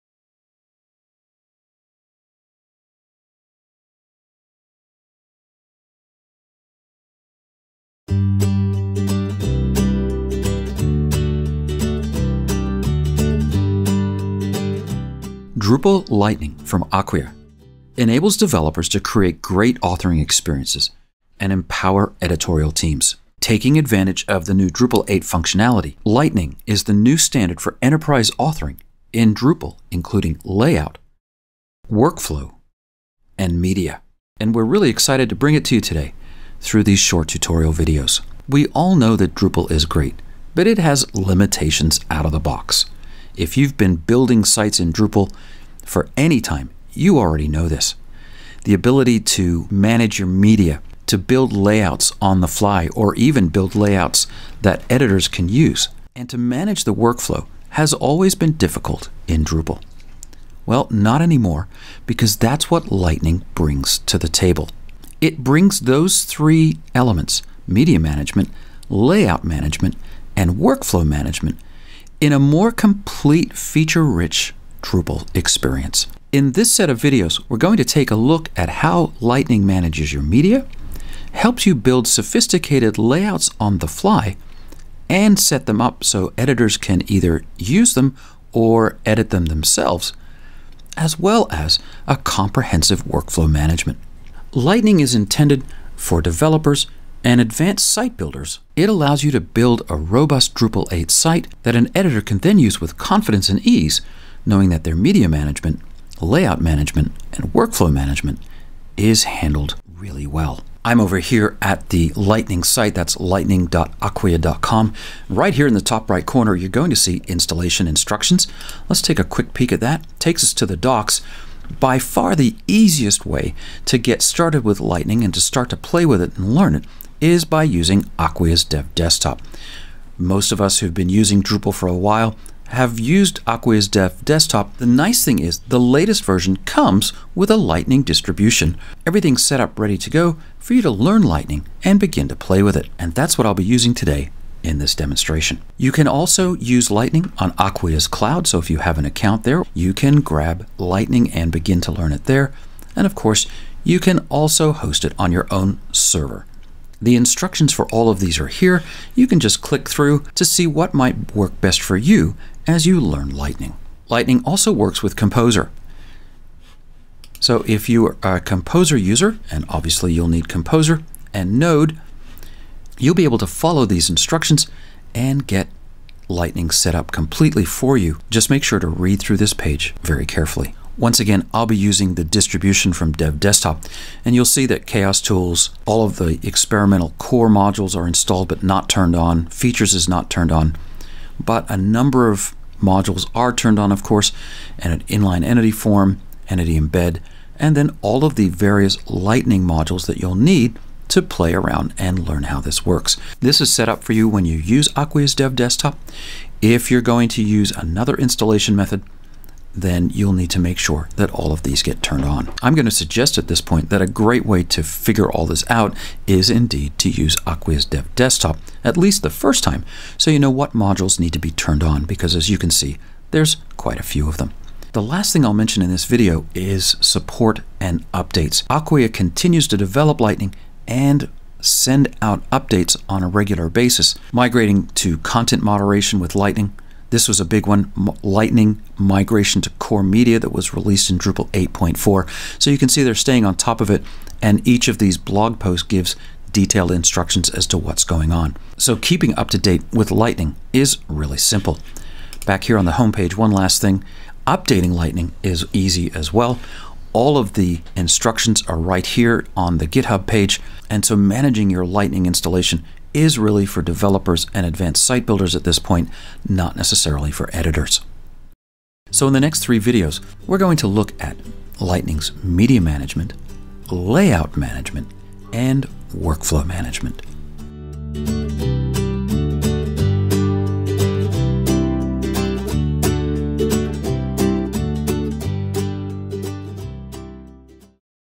Introduction To Lightning: Overview
Lightning is a Drupal distribution from Acquia enabling developers to build sites and digital experiences with Drupal 8 quicker.

The intent of Lightning is to enable developers to create great authoring experiences and empower editorial teams. We do this by providing a lightweight framework, documentation and best practice examples for building working solutions in Drupal.

In Drupal 8, we have taken advantage of new D8 functionality and tightly coupled functional areas we consider to make up the new standard for enterprise authoring in Drupal including Layout, Preview, Workflow and Media. We have also embedded hundreds of automated tests allowing developers to implement continual integrations pipelines that monitor major functionality, essentially providing a safe environment to innovate with their own custom code additions to Lightning.